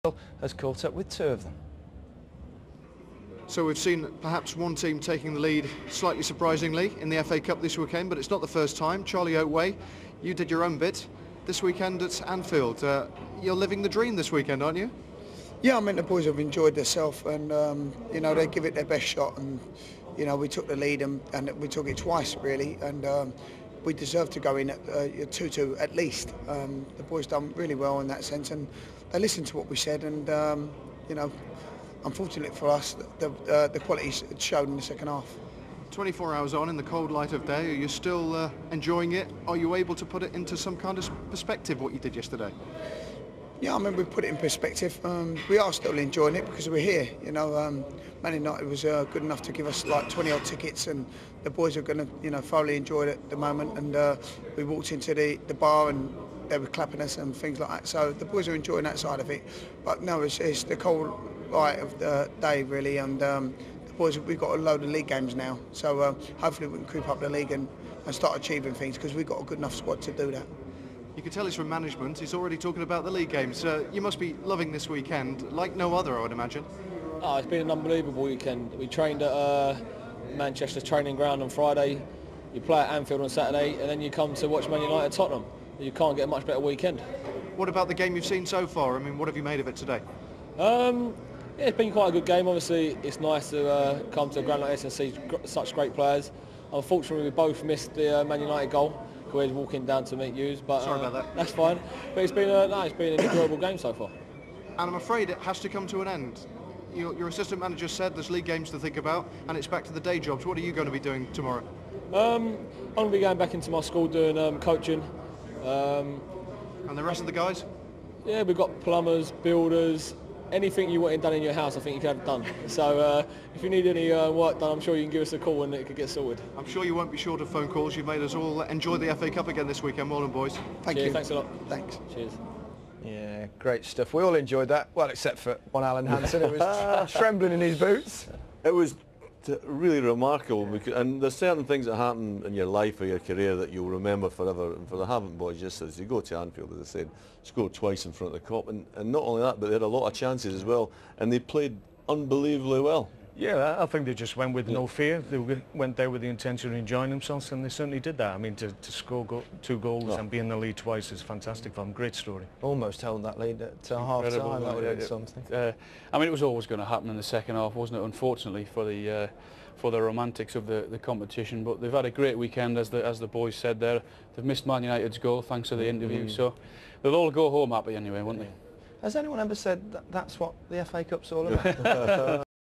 Charlie Oatway & Jamie Collins Interview
Charlie Oatway and Jamie Collins of Havant & Waterlooville are interviewed by Ivan Gaskell of BBC during half-time of the Manchester United vs Tottenham Hotspur match. They and the squad were given 20 odd tickets for the game from Manchester United. Ivan asks them how the game was and Jamie says its been quite a good game but unfortunately they missed the goal when going down to meet Ivan. What a waste hey!